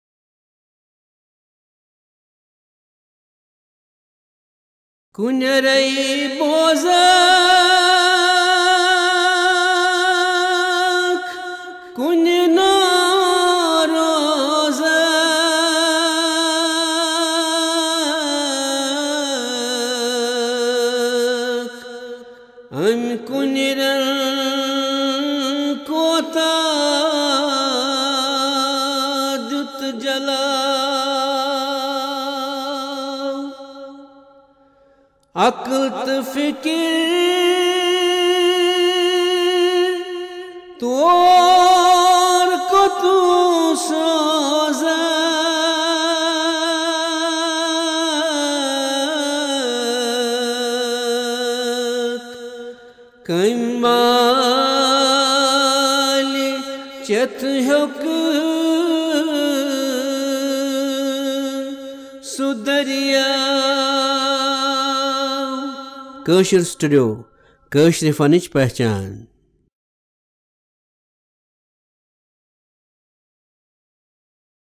Kashur Studio: Channel Trailer by Munir Ahmad Mir
Kashur Studio aims to feature the best of Kashmiri music by bringing together some of the finest singers, musicians and composers and giving them a stage to celebrate Kashmir's rich musical heritage.

Kashur Studio is an independent platform--based in Srinagar spotting some of the finest and young musicians. It has remained a remarkable musical journey so far and it Kashur Studio continues to dig deep into finding hidden talent and celebrating with the people across the world.

Label - M.Tech Studio, Srinagar